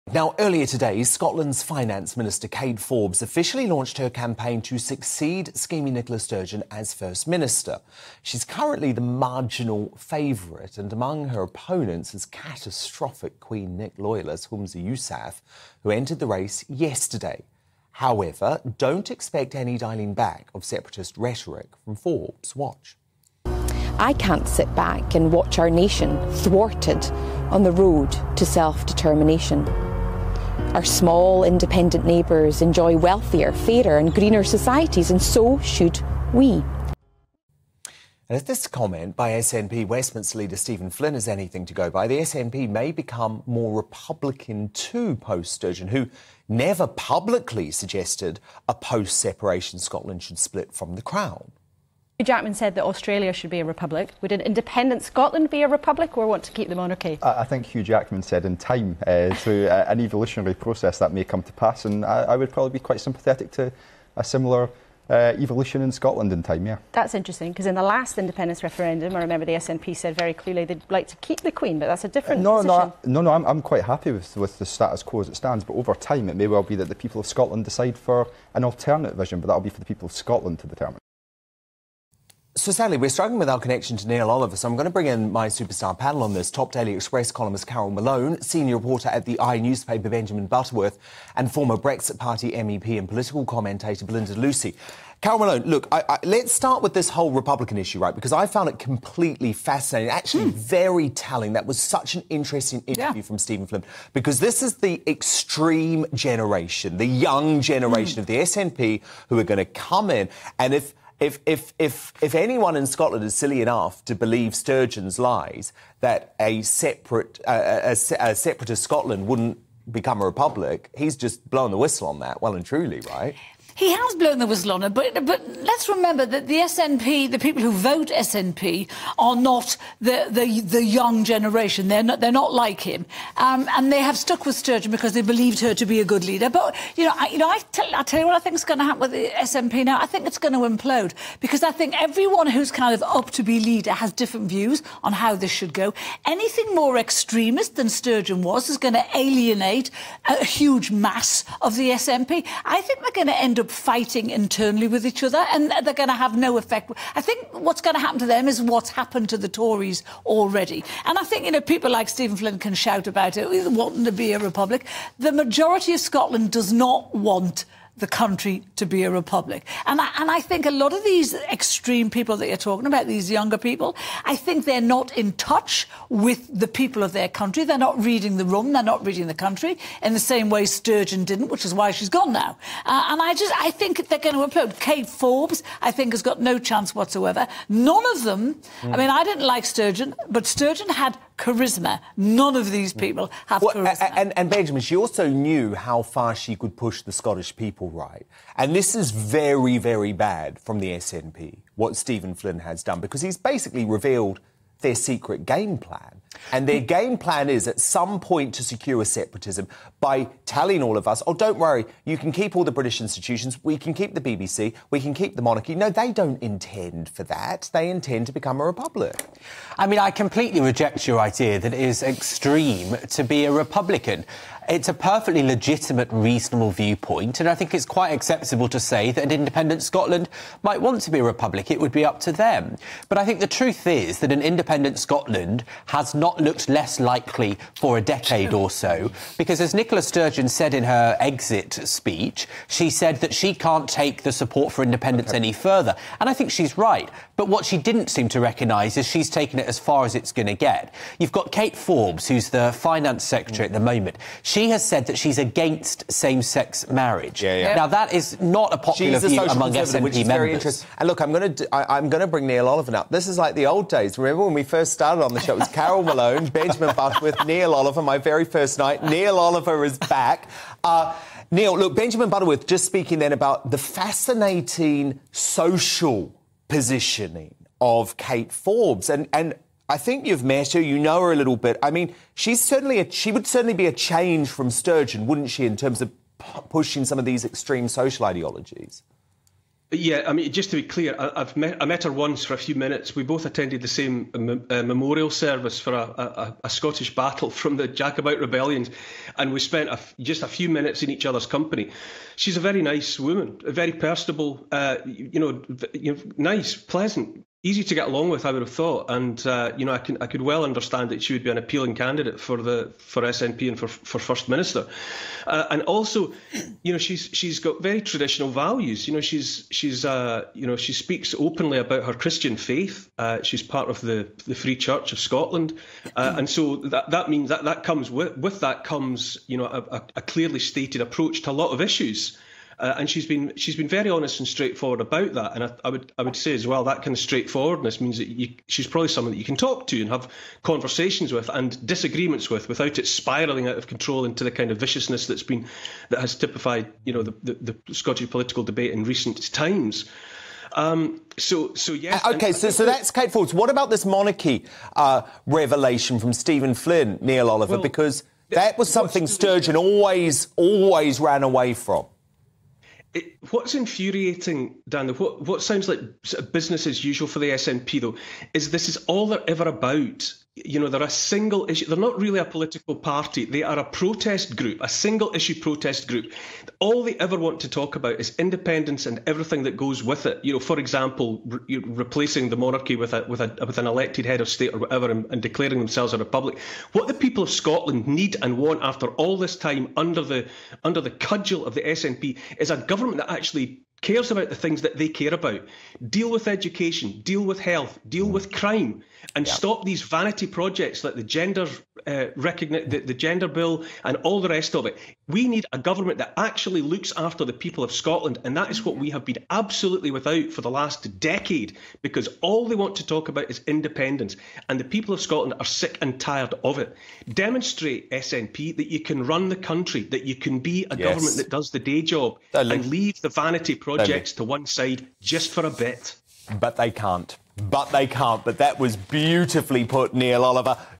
Neil Oliver: The SNP needs to deal with education, health, and crime, and stops these vanity project
Neil Oliver: What the people of Scotland need and want after all this time under the cudgel of the SNP is a government that actually cares about what the people care about. That deals with education, health, and crime, and stops these vanity projects.